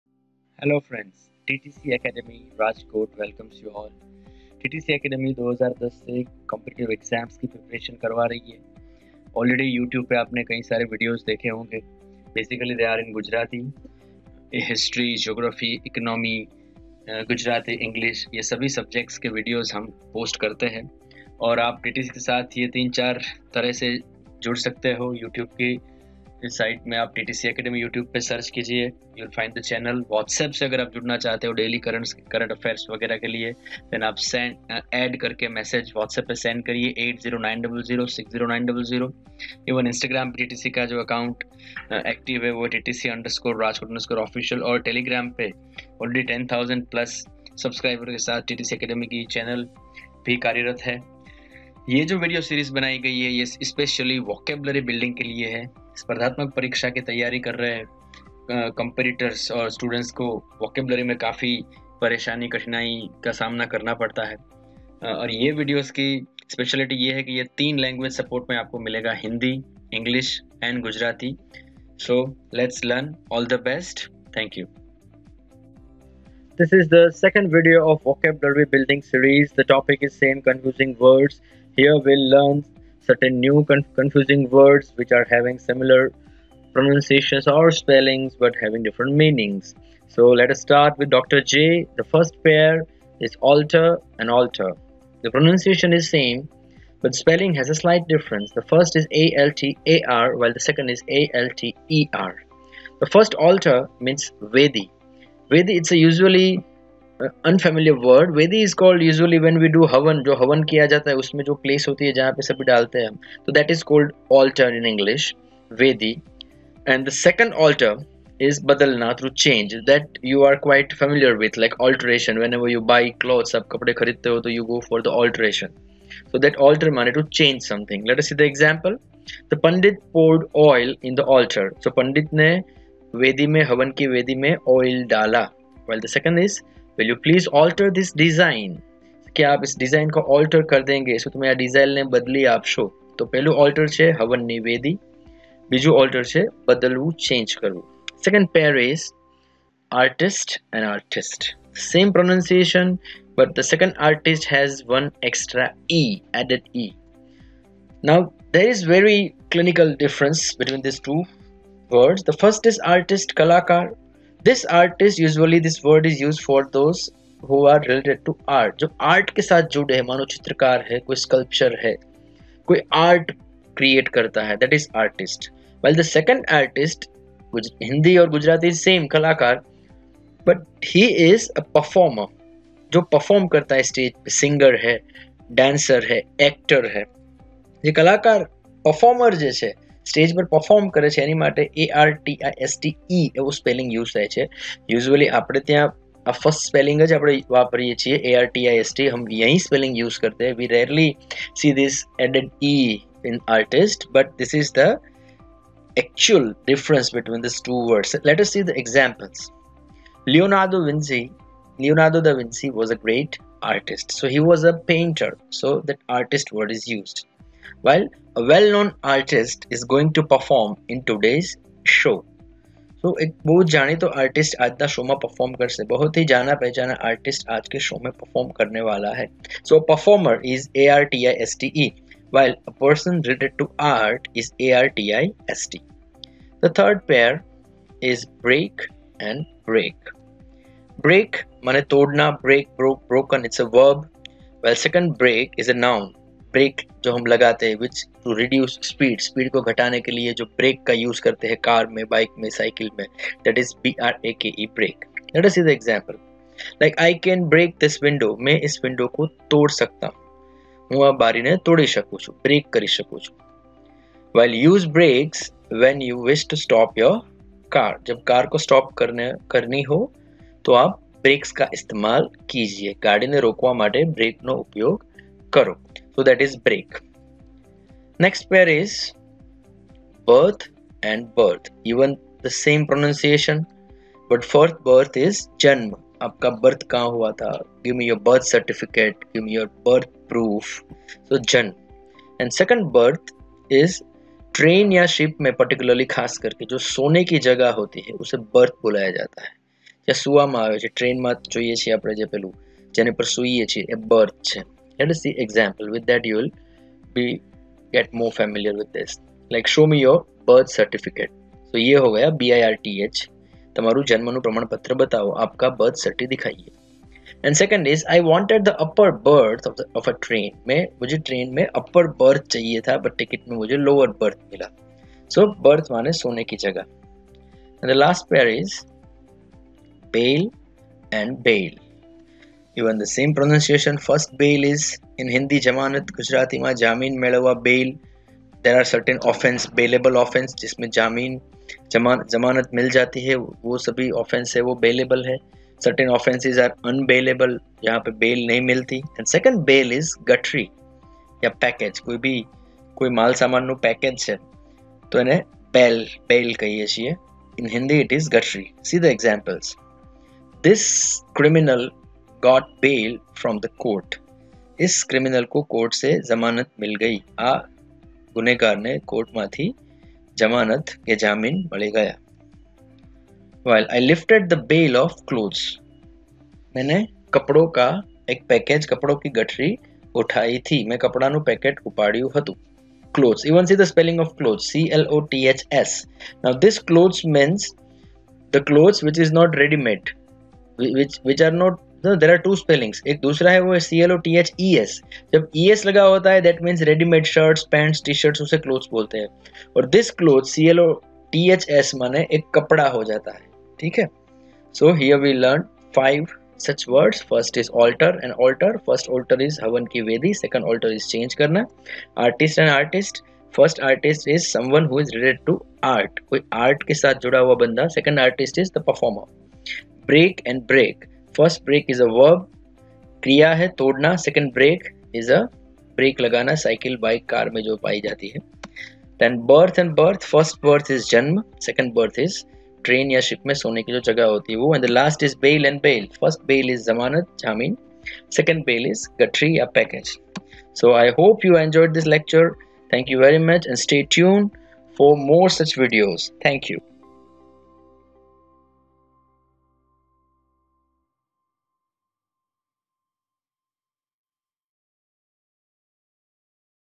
ENGLISH VOCABULARY PART 2 CONFUSING WORDS, HOMONYMS, HOMOPHONES
The Video is about confusing English Words. The Homonyms, Homophones and Homographs. The Video has English, Hindi and Gujarati language Support.